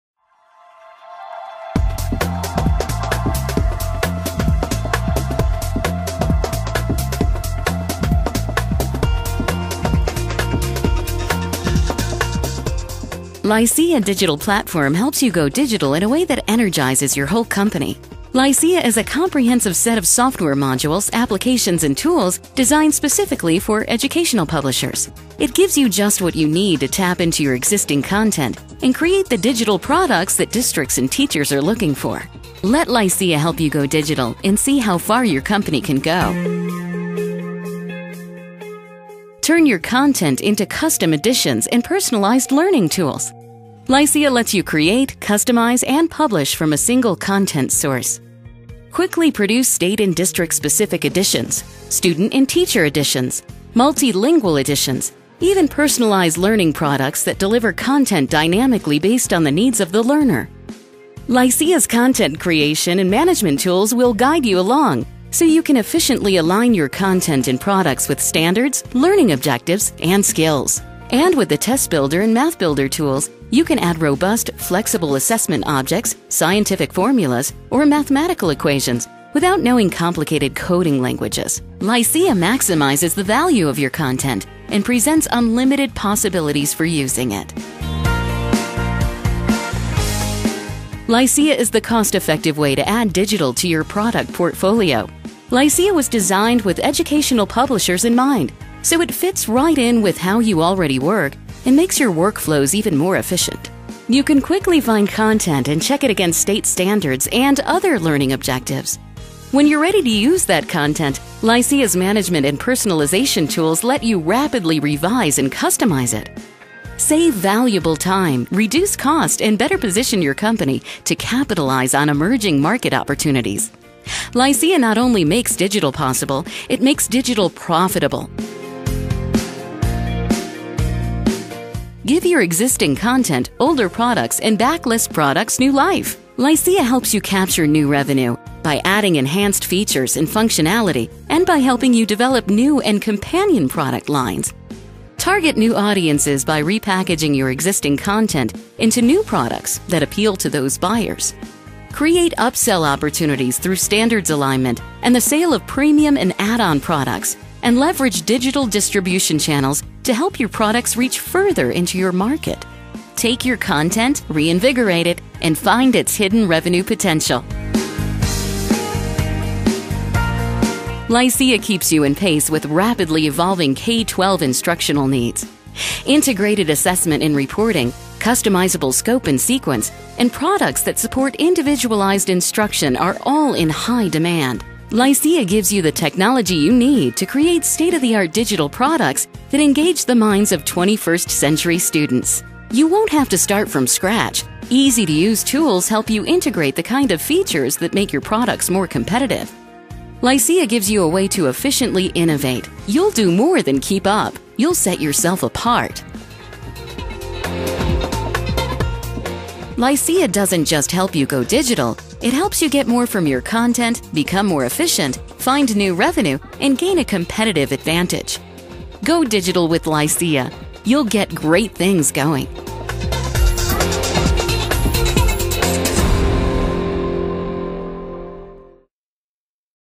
Lycea Digital Platform Overview
Digital comes with a lot of questions.  Lycea Digital Platform lets you turn the challenge of going digital into opportunities.  Designed specifically for educational publishers, it has the tools you need to find your way in the evolving digital marketplace.  Find your answers with Lycea.